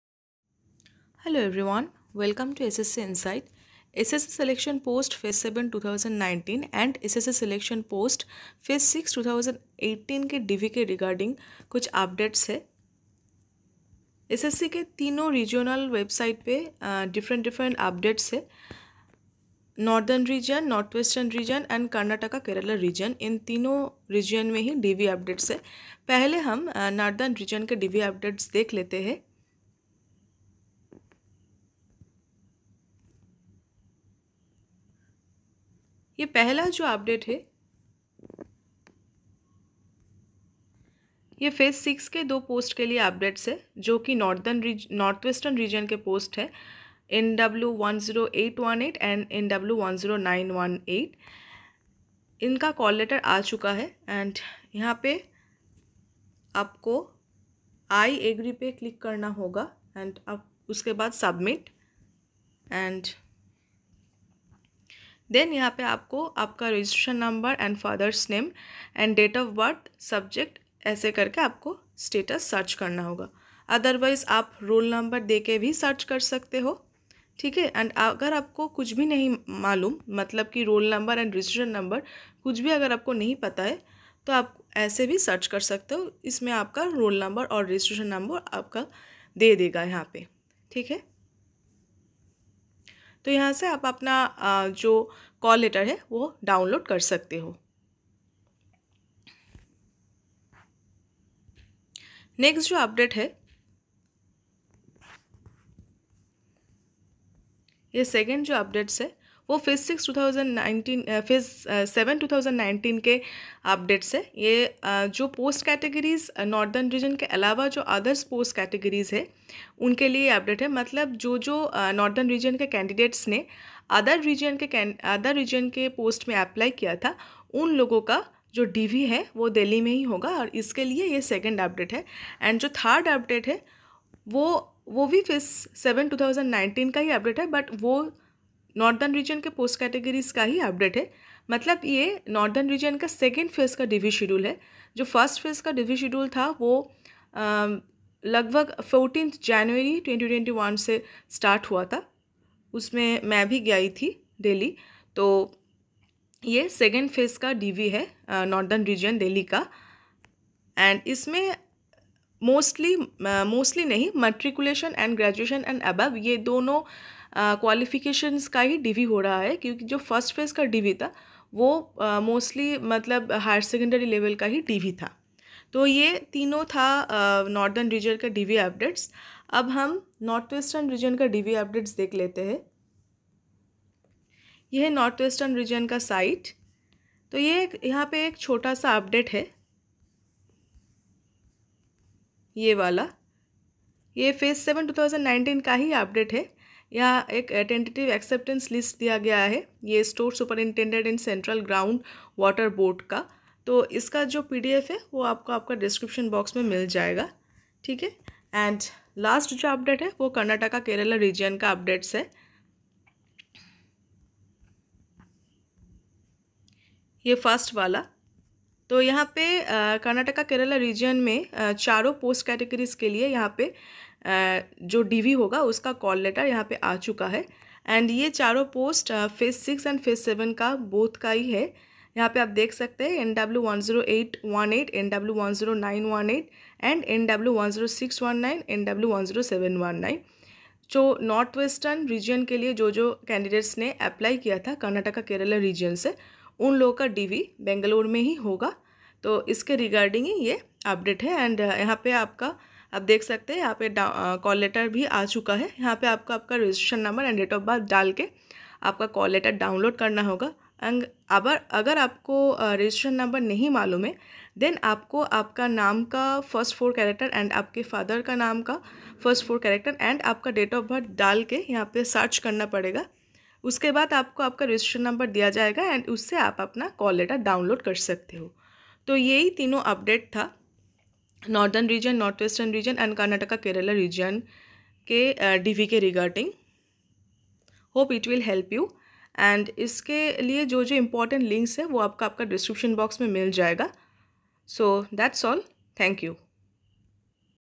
DOWNLOAD DV CALL LETTER | IN NR NWR & KKR | SSC SELECTION POST PHASE VII/2019
CALL LETTER FOR THE DOCUMENT VERIFICATION OF SELECTION POST EXAMINATION-PHASE-VI/2018 FOR THE POST CATEGORIES I.E. NW10818 & NW10918 OF NWR REGION:

CALL LETTER FOR THE DOCUMENT VERIFICATION OF SELECTION POST EXAMINATION-PHASE-VII/2019 FOR THE POST CATEGORIES OF OTHER THAN NORTHERN REGION:

CALL LETTER FOR THE DOCUMENT VERIFICATION OF SELECTION POST EXAMINATION-PHASE-VII/2019 FOR THE POST CATEGORIES OF NORTHERN REGION:

TENTATIVE EQ ACCEPT LIST FOR THE POST OF STORE SUPERINTENDENT IN CENTRAL GROUND WATER BOARD, FARIDABAD ADVERTISED VIDE POST CODE NO. NW10919 OF ADVERTISEMENT NO.PHASE VII/2019-SELECTION POSTS

DOWNLOAD CALL LETTER OF KKR